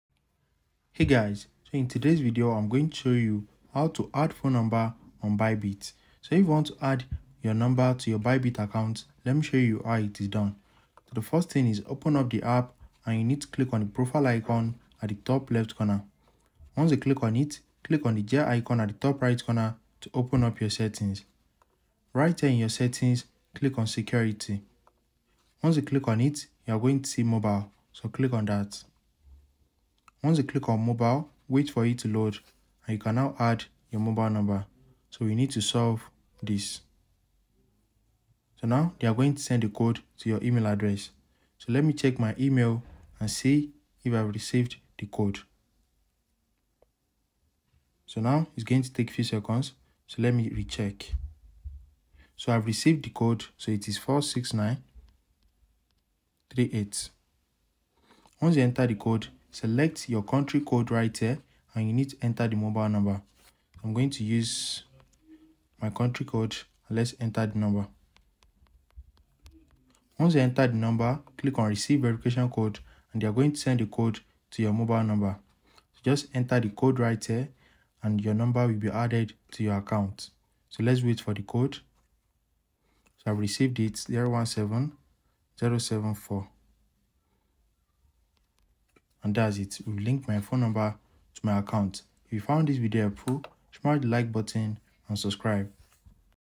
How to Add Phone Number on Bybit
Want to enhance your ByBit account security? Learn how to add a phone number to your account for two-factor authentication and account recovery. This video will guide you through the process of verifying your phone number and securing your ByBit account.